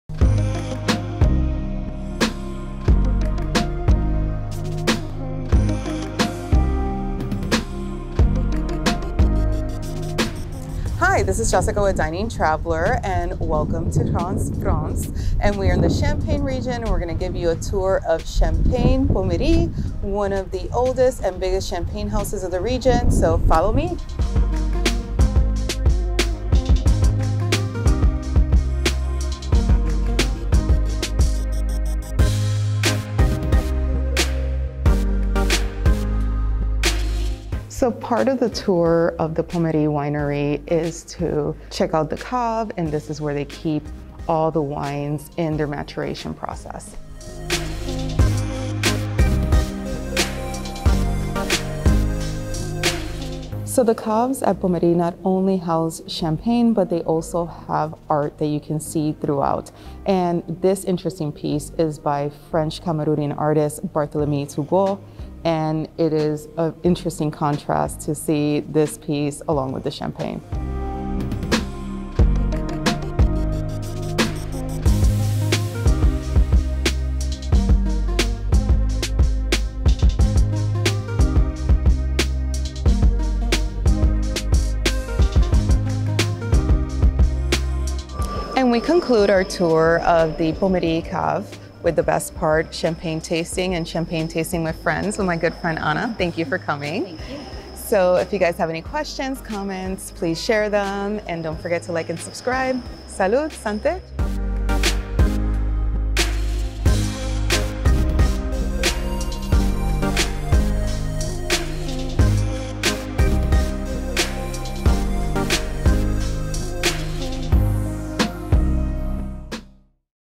Tour of Champagne Pommery in Reims, France
No trip to Reims is complete without touring Champagne wineries. From small independent producers to big champagne houses, there’s plenty to be sampled in the region. A tour of Champagne Pommery, one of the oldest and largest champagne houses is a must.

Go for the champagne, but stay for the art! We took a self-guided tour which led us through the caves where the champagne undergoes its maturation process. Keep your eyes open and explore the many art installations from artists from France and beyond. End the visit with a perfectly poured glass of Pommery.

I enjoy exploring both large and small producers when visiting a wine region. It gives you an interesting perspective. What do you like to do when visiting a wine region?